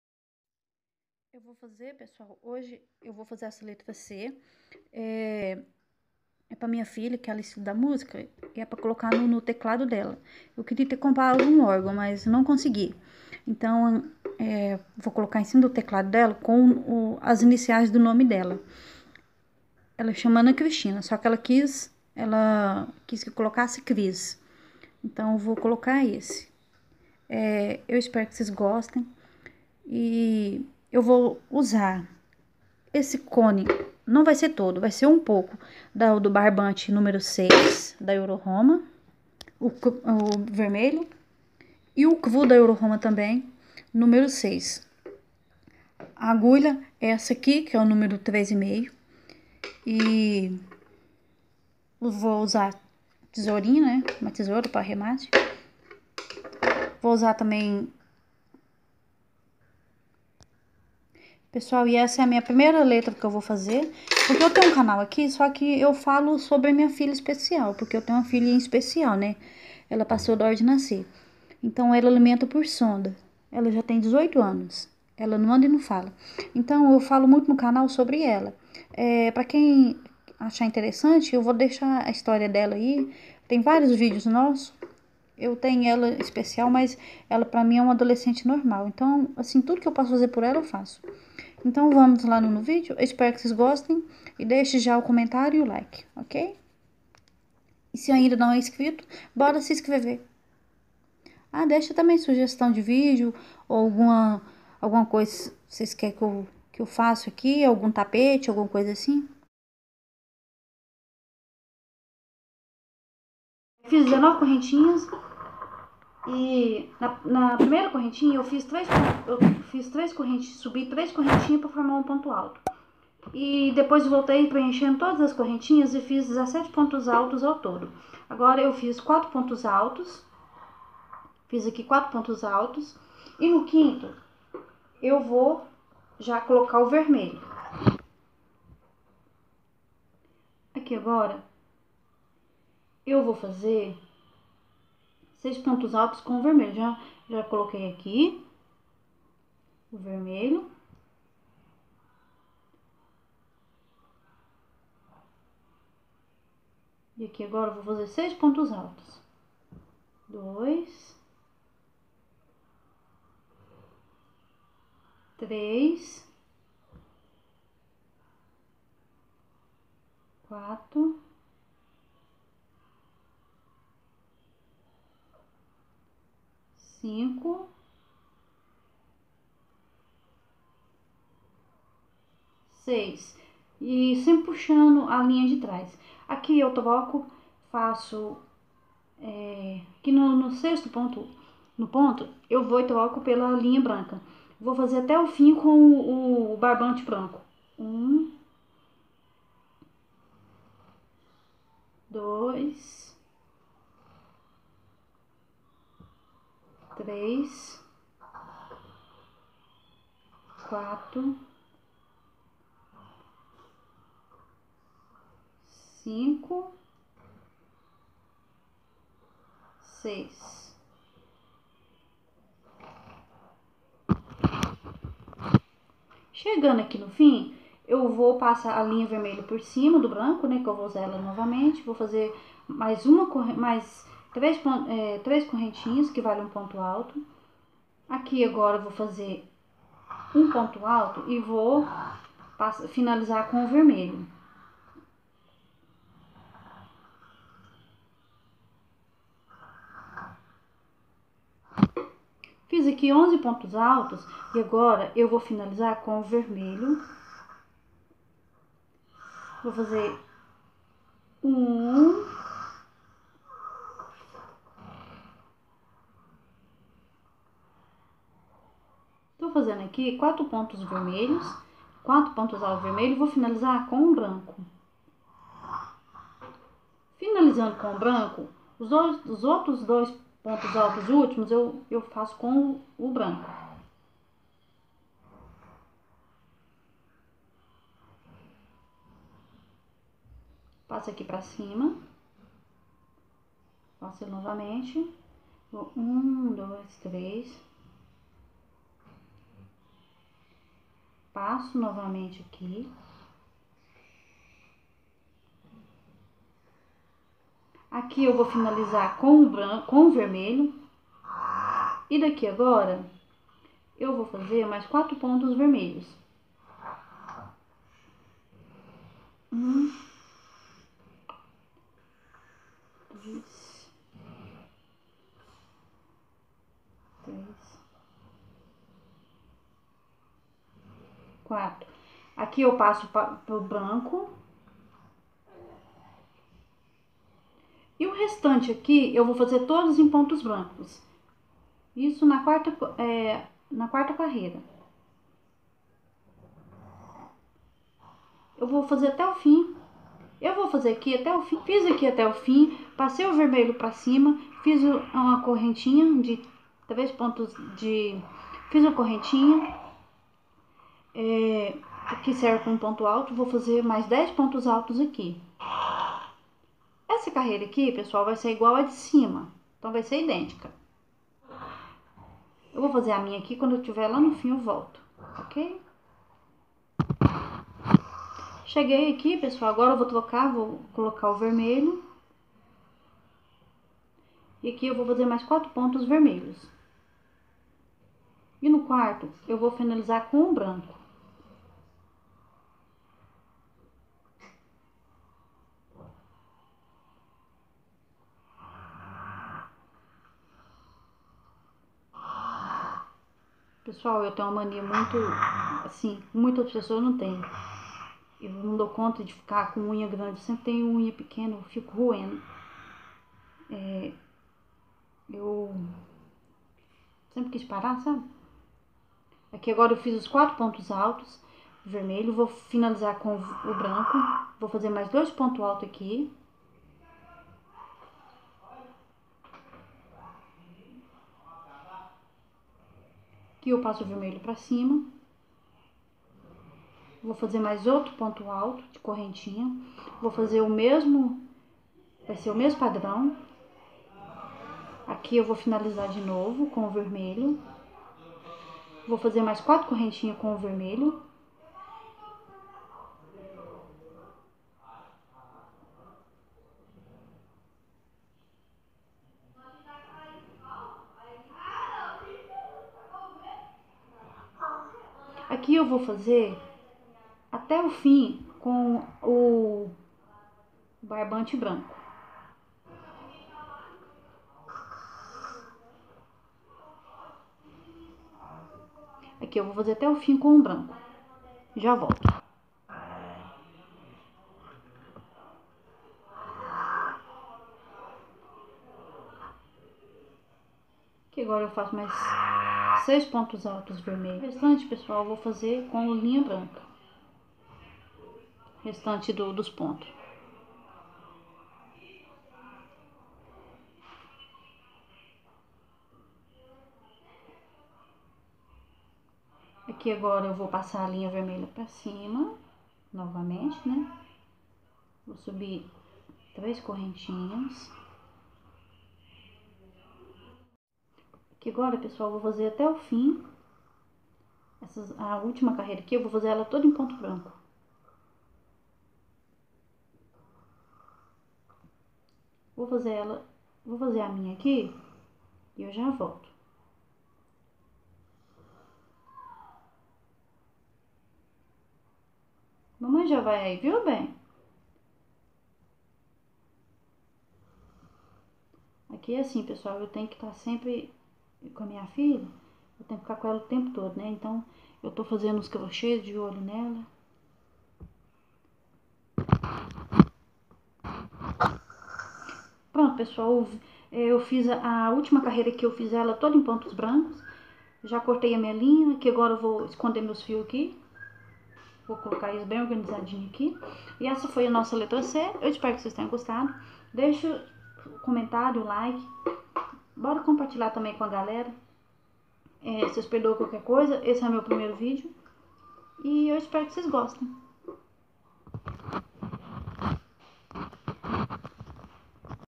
Alfabeto em ponto conduzido
Olá galera tudo bem...Hoje vou fazer minha primeira vídeo aula de croché, mostro pra vocês como confeccionar a letra C em fio conduzido. ..pra quem não sabe sou mãe de uma mocinha com paralisia cerebral que não anda e nem fala .E não posso trabalhar, aprendi o croché em um projeto na minha cidade. ..espero que gostem

Lista dos materiais que estou usando
Barbante número 6 euroroma cor cru
Barbante número 6 euroroma cor vermelha
Agulha número 3,5
Tesoura pra arremates